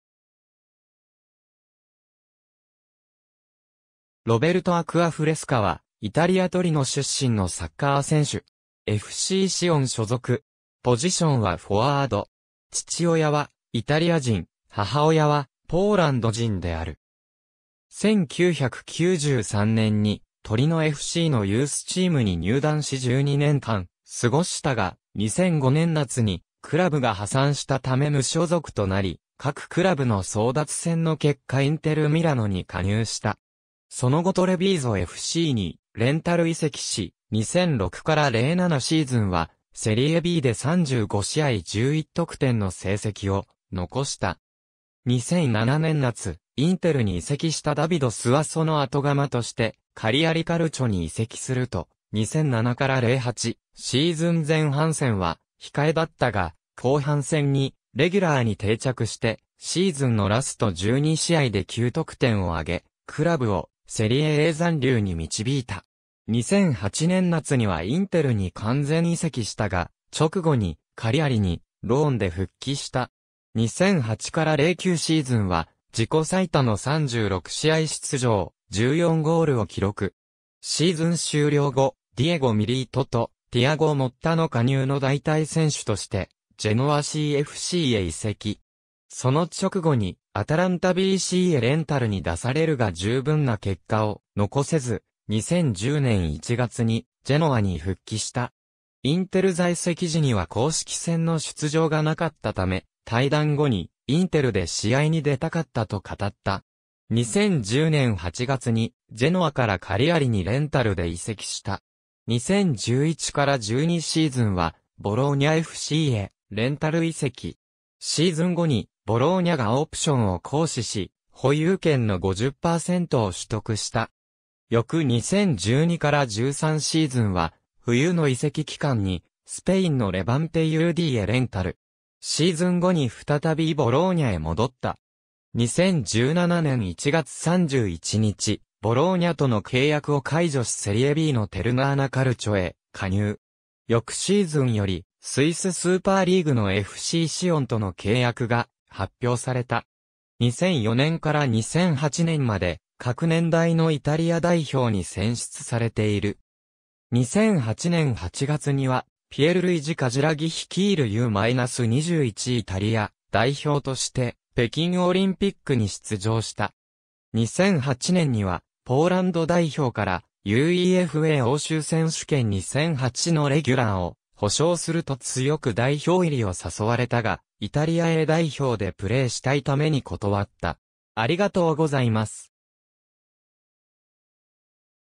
ロベルト・アクアフレスカ
テルナーナ・カルチョの選手
ロベルト・アクアフレスカ（Robert Acquafresca, 1987年9月11日 - ）は、イタリア・トリノ出身のサッカー選手。
FCシオン所属。
父親はイタリア人、母親はポーランド人である。
1993年にトリノFCのユースチームに入団し12年間過ごしたが、2005年夏にクラブが破産したため無所属となり、各クラブの争奪戦の結果インテル・ミラノに加入した。
その後トレヴィーゾFCにレンタル移籍し、2006-07シーズンはセリエB（2部）で35試合11得点の成績を残した。
2007年夏、インテルに移籍したダビド・スアソの後釜としてカリアリ・カルチョに移籍すると、2007-08シーズン前半戦は控えだったが、後半戦にレギュラーに定着してシーズンのラスト12試合で9得点を挙げ、クラブをセリエA残留に導いた。
2008年夏にはインテルに完全移籍したが、直後にカリアリにローンで復帰した。
2008-09シーズンは自己最多の36試合出場、14ゴールを記録。
シーズン終了後、ディエゴ・ミリートとティアゴ・モッタの加入の代替選手としてジェノアCFCへ移籍。
その直後にアタランタBCへレンタルに出されるが十分な結果を残せず、2010年1月にジェノアに復帰した。
インテル在籍時には公式戦の出場がなかったため、退団後に「インテルで試合に出たかった」と語った。
2010年8月にジェノアからカリアリにレンタルで移籍した。
2011-12シーズンはボローニャFCへレンタル移籍。
シーズン後にボローニャがオプションを行使し、保有権の50％を取得した。
翌2012-13シーズンは、冬の移籍期間にスペインのレバンテUDへレンタル。
シーズン後に再びボローニャへ戻った。
2017年1月31日、ボローニャとの契約を解除しセリエBのテルナーナ・カルチョへ加入。
翌シーズンよりスイス・スーパーリーグのFCシオンとの契約が発表された。
2004年から2008年まで各年代のイタリア代表に選出されている。
2008年8月にはピエルルイジ・カジラギ率いるU-21イタリア代表として北京オリンピックに出場した。
2008年にはポーランド代表から「UEFA欧州選手権2008のレギュラーを保証する」と強く代表入りを誘われたが、イタリアA代表でプレーしたいために断った。